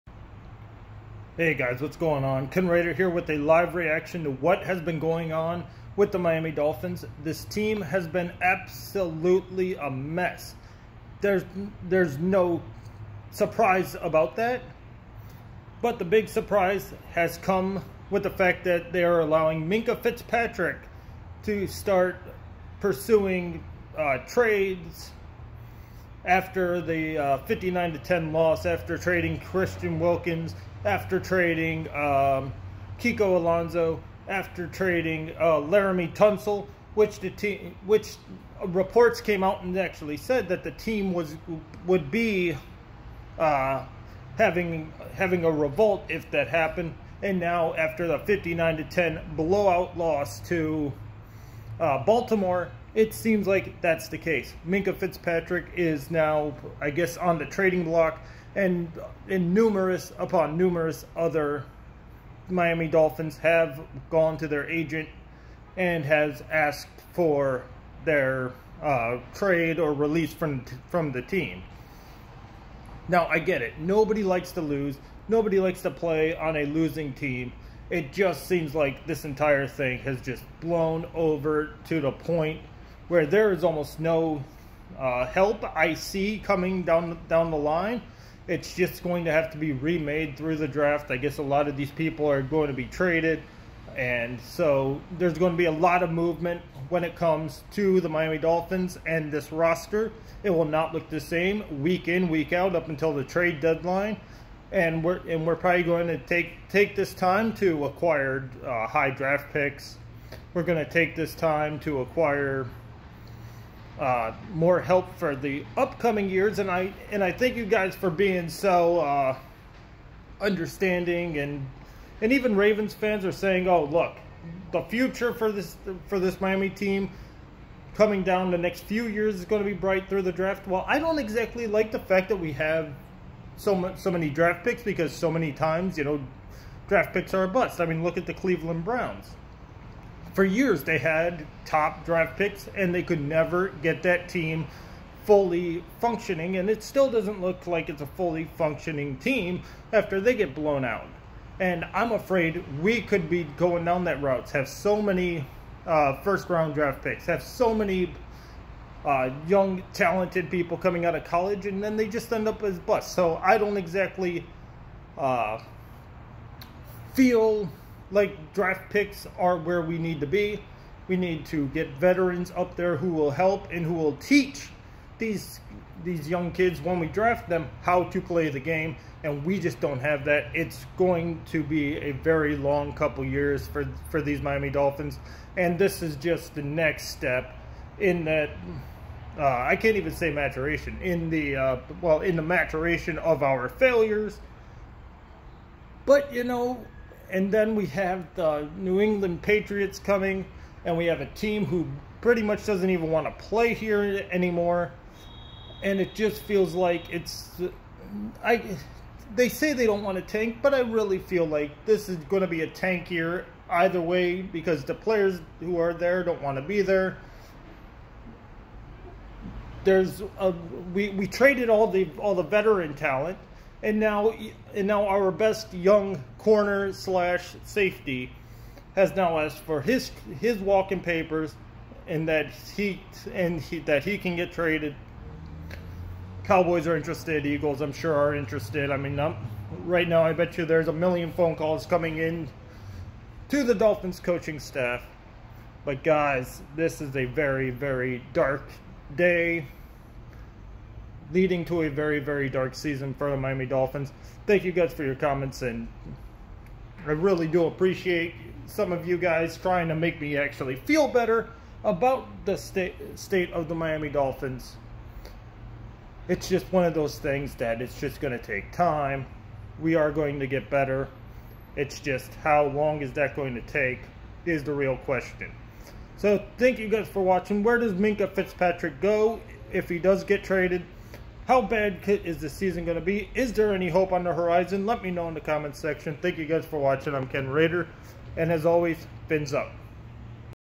Dolphins to allow Minkah Fitzpatrick to pursue trade out of Miami... FinsUp in turmoil
MinkahFitzpatrick has been granted his request to explore Trade options after he and other Dolphins players have expressed interest on leaving the team